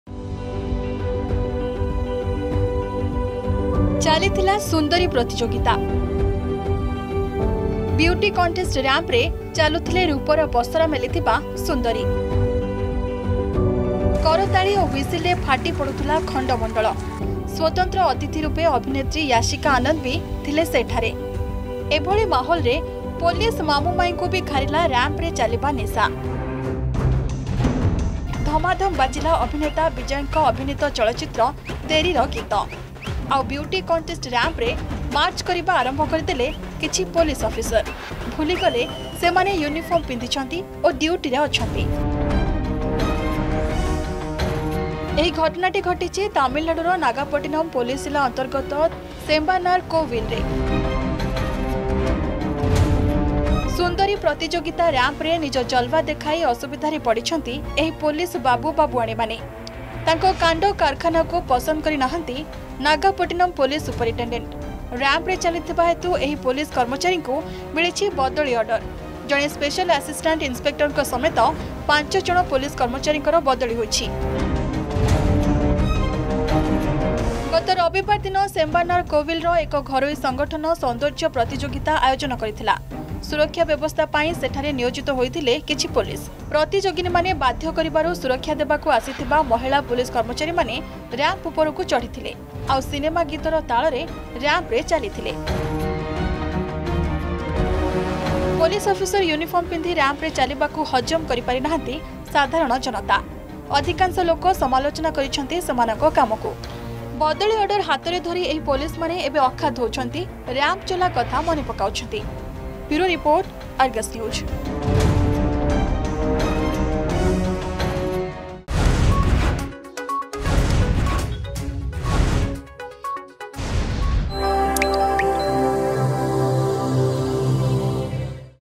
ରେଡ୍ କାର୍ପେଟରେ ଖାକି
ଚାଲିଥିଲା ସୁନ୍ଦରୀ ପ୍ରତିଯୋଗିତା । ବିଉଟି କନଟେଷ୍ଟ ରାମ୍ପରେ ଚାଲୁଥିଲେ ରୂପର ପସରା ମେଲିଥିବା ସୁନ୍ଦରୀ । ଏଭଳି ମାହୋଲରେ ପୋଲିସ ମାମୁଁ ମାଇଁଙ୍କୁ ବି ଘାରିଲା ରାମ୍ପରେ ଚାଲିବା ନିଶା । ଆଉ ବିଉଟି କନଟେଷ୍ଟ ରାମ୍ପରେ ମାର୍ଚ୍ଚ କରିବା ଆରମ୍ଭ କରିଦେଲେ କିଛି ପୋଲିସ ଅଫିସର । ଭୁଲି ଗଲେ ସେମାନେ ୟୁନିଫର୍ମ ପିନ୍ଧିଛନ୍ତି ଓ ଡ୍ୟୁଟିରେ ଅଛନ୍ତି ।